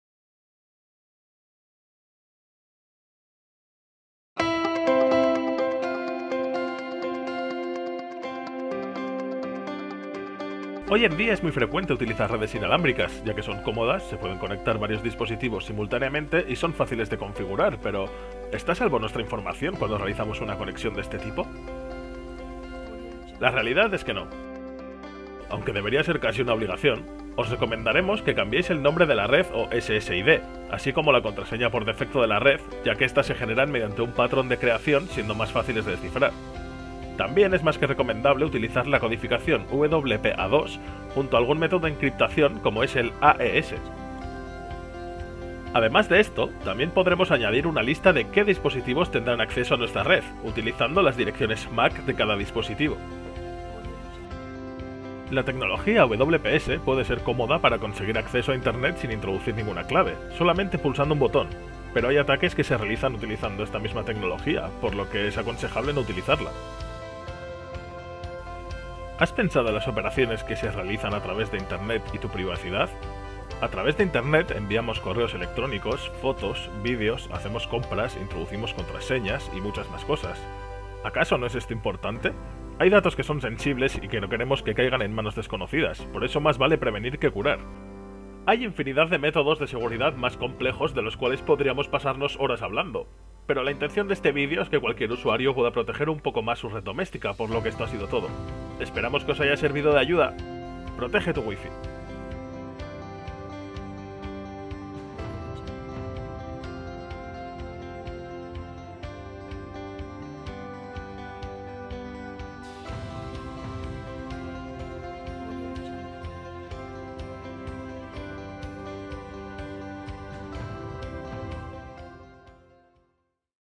Cómo mantener tu Wi-Fi segura
Si tienes dudas de como hacer tu red Wi-fi más segura, aquí tienes unos consejillos fáciles de aplicar en tu red doméstica.